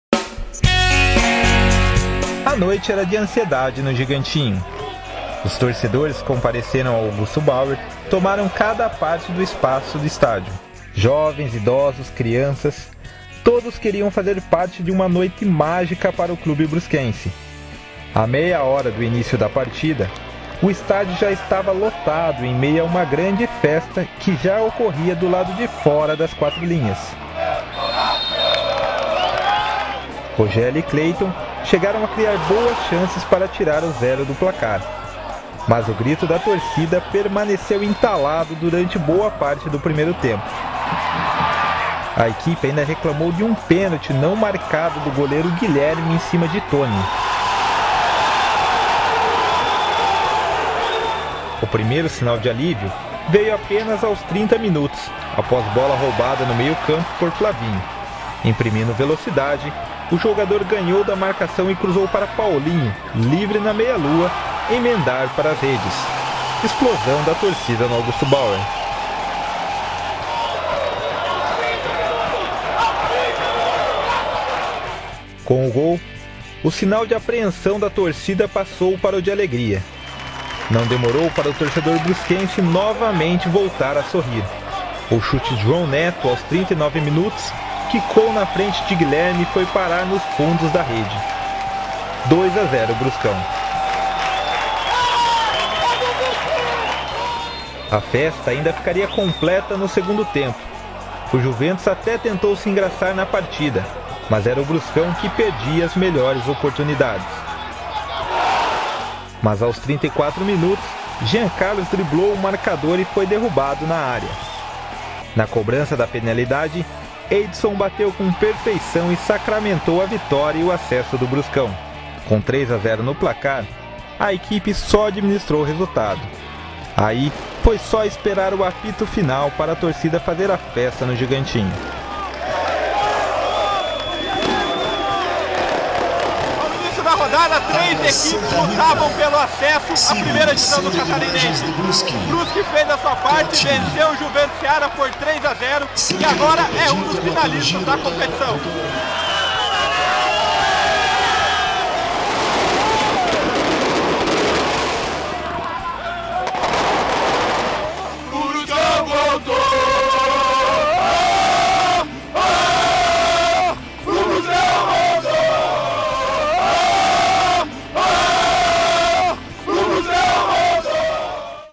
Bruscão vence Juventus Seara e celebra retorno à primeira divisão
Com Augusto Bauer lotado, o Marreco bateu o Juventus Seara na noite de segunda-feira, 17 de agosto, e carimbou o retorno à elite do Campeonato Catarinense em 2016.